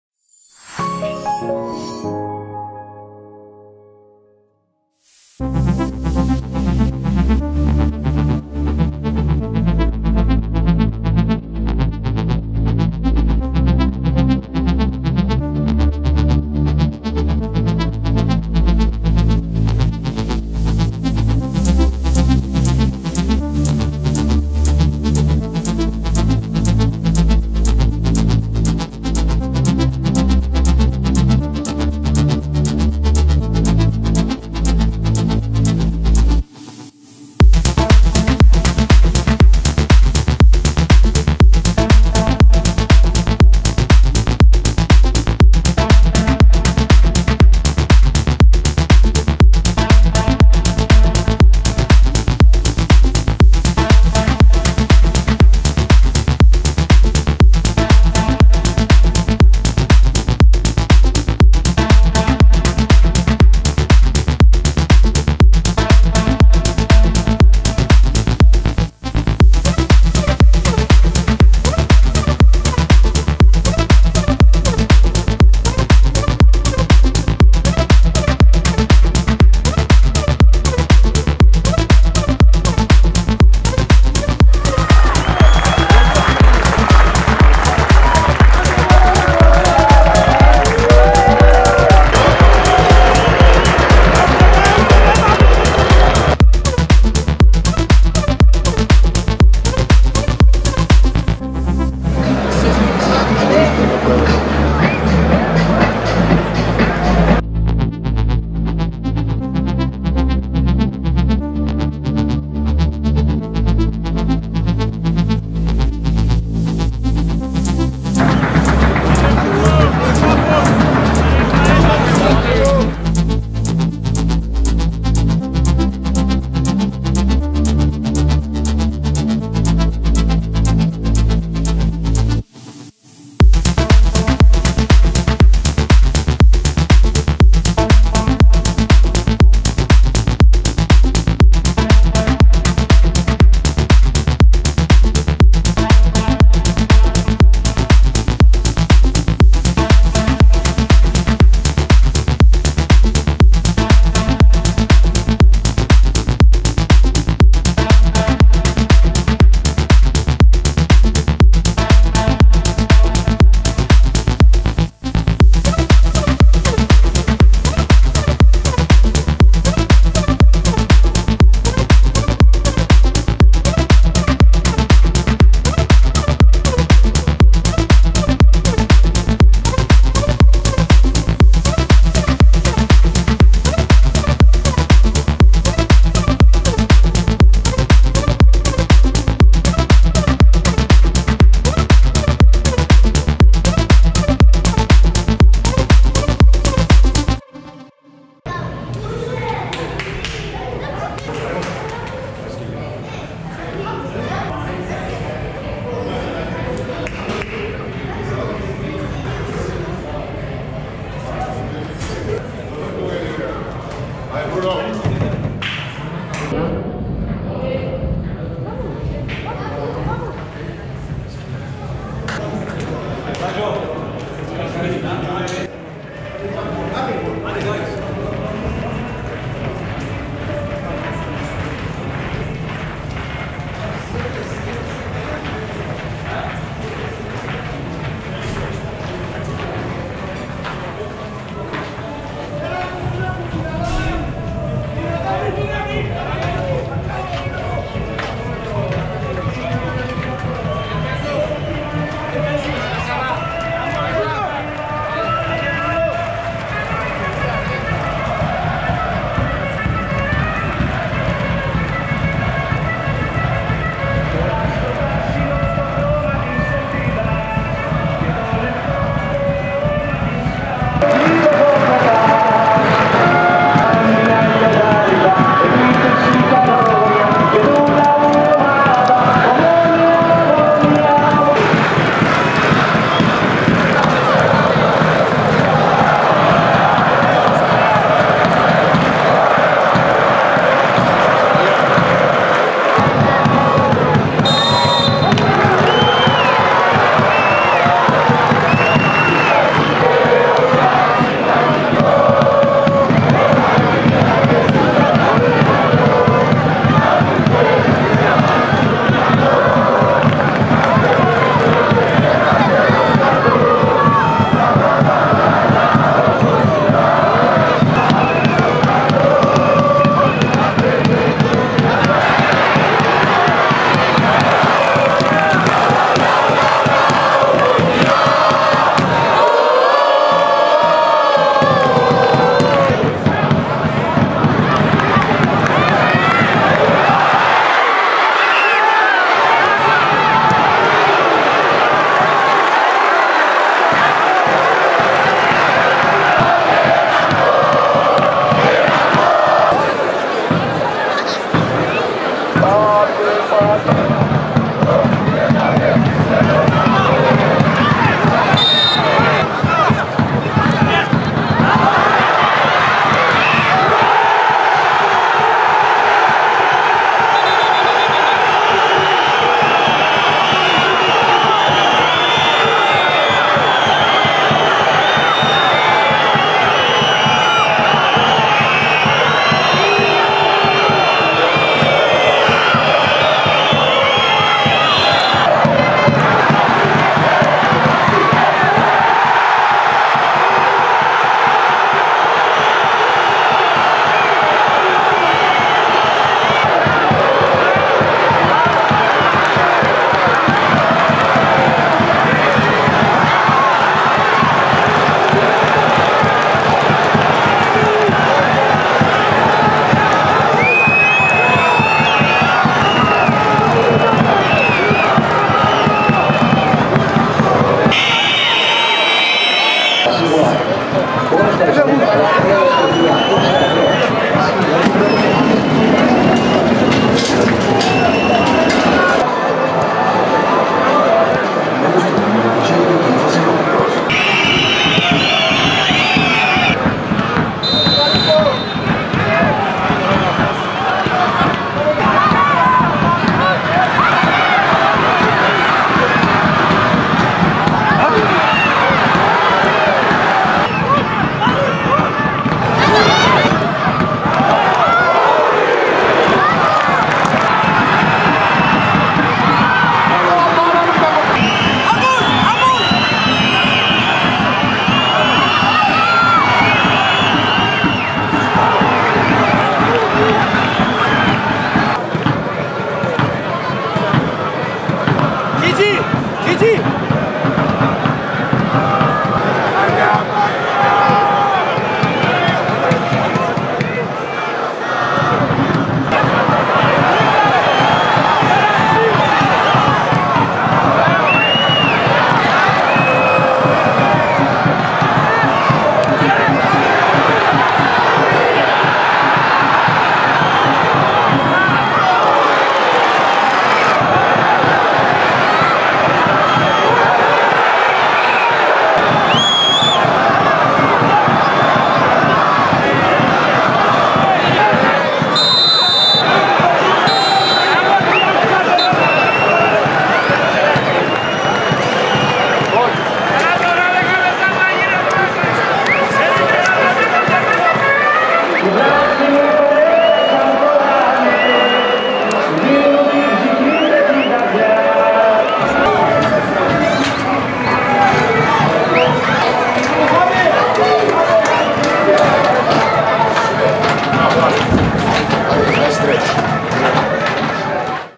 Παρακάμερα | ΟΜΟΝΟΙΑ - Παφος FC 0-0 (21η αγ. πρωταθλήματος 2022-23)
Η παρακάμερα του ψεσινού αγώνα απέναντι στην Πάφος FC 🎥

ΑΚΟΛΟΥΘΕΙΣΤΕ ΜΑΣ!